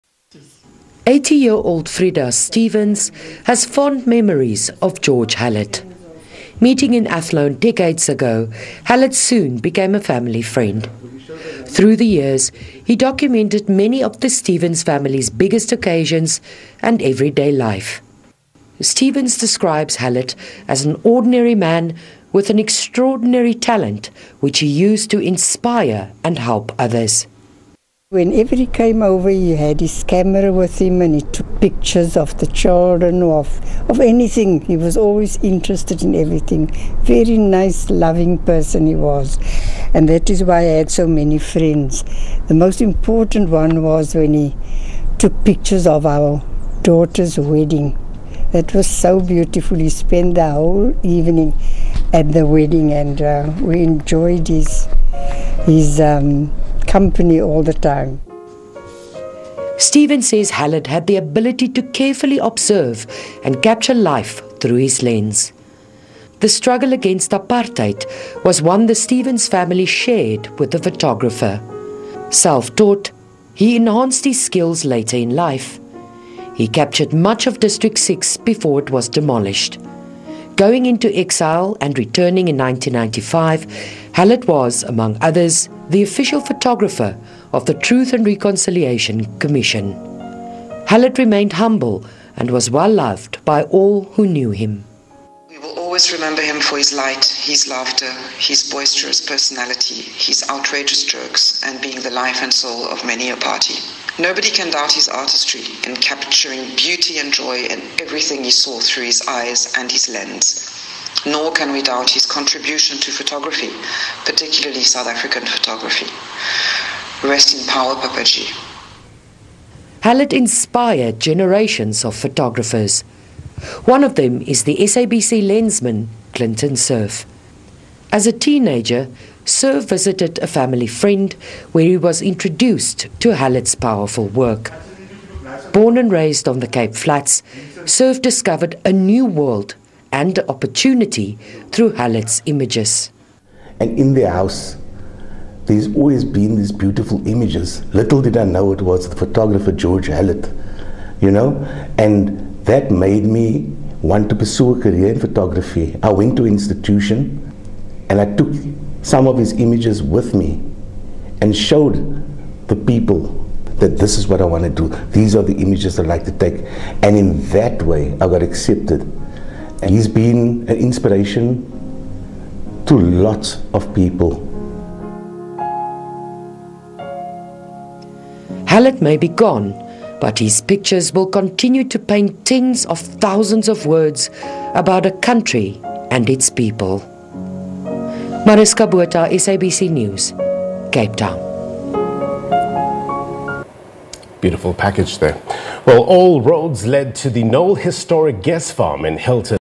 World-renowned photographer George Hallet remembered
World-renowned photographer George Hallet passed away last week in Cape Town.
Hallet is well known for documenting life under apartheid and the early days of democracy in South Africa.
The 78-year-old photographer died peacefully in his sleep after a long illness...